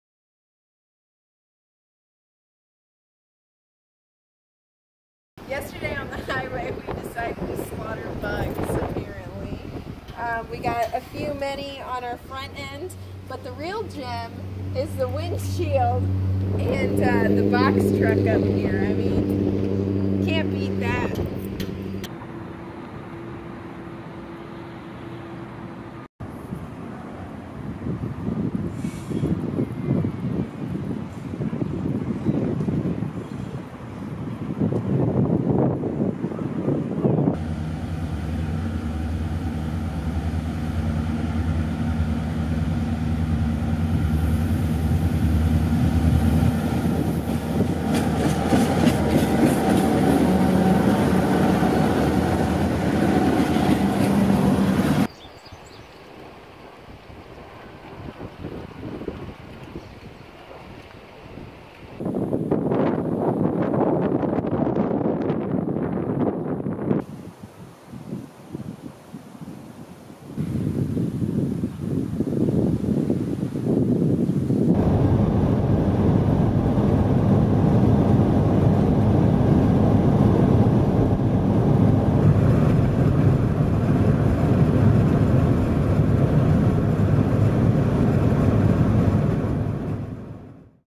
Day 8: Travel Through Midwest | West 2019
So we got stuck.  Then unstuck.  Then killed a bunch of bugs.  Then stopped in Green Bay, then drove through some great farmland in Wisconsin and Minnesota, and even fit in some bouldering.

Drop a line below.